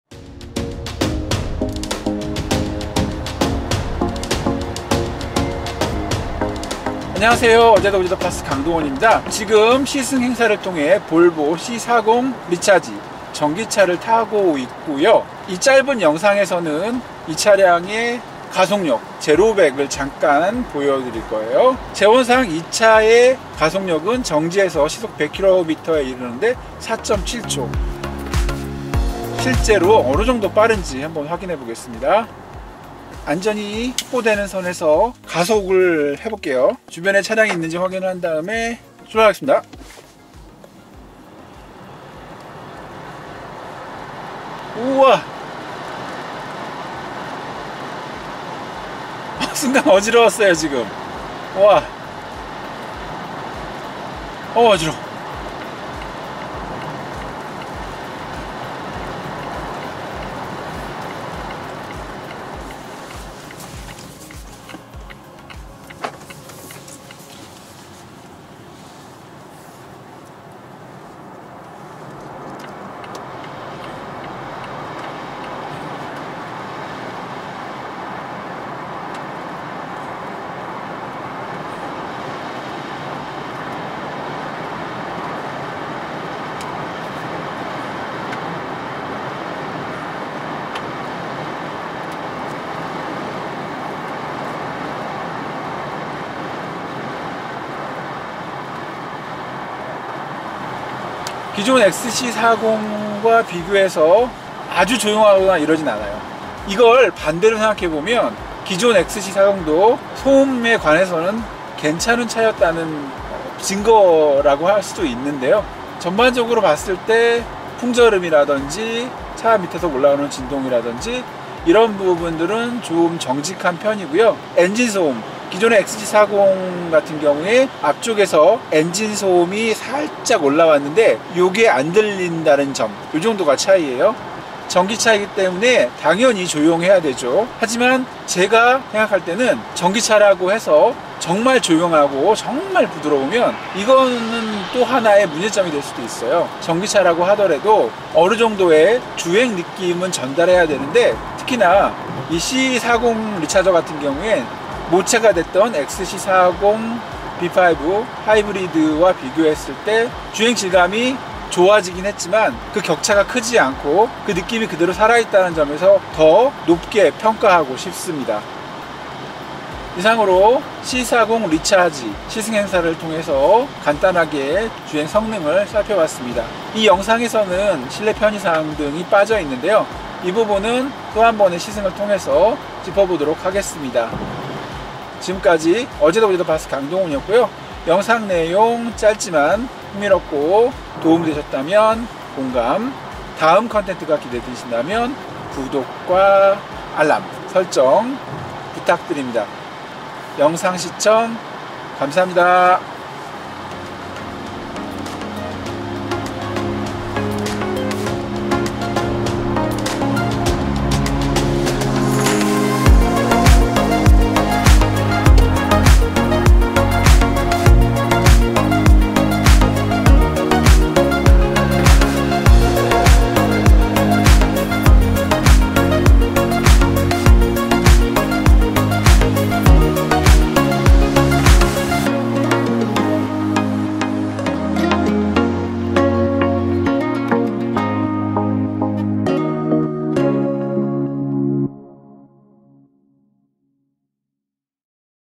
볼보 C40 리차지 AWD 순수전기차 제로백 · 고속주행
_ 본 영상에서 소개되는 C40 리차지 트윈 얼티메이트는 볼보자동차 코리아로부터 제공받은 시승차입니다.

볼보자동차의 순수전기 쿠페형 SUV C40 리차지의 고속주행, 승차감, 주행성능, 가속 성능을 확인해봤습니다.